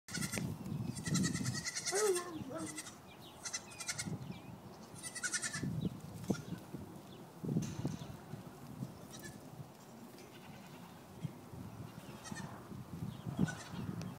Armenian Gampr Zoey with her goats and chickens
Zoey is owned by Judy Casey and lives near Los Angeles, guarding some goats and chickens.